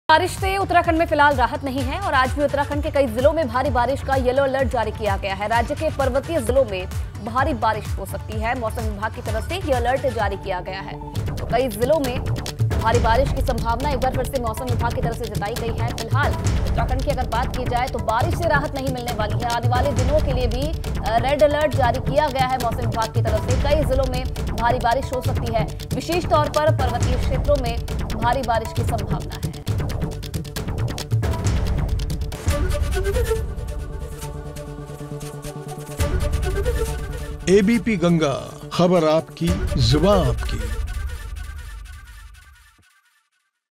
Uttarakhand Breaking: उत्तराखंड के कई जिलों में भारी बारिश का येलो अलर्ट
Uttarakhand Breaking: उत्तराखंड के कई जिलों में भारी बारिश का येलो अलर्ट, राज्य के पर्वतीय जिलों में भारी बारिश की चेतावनी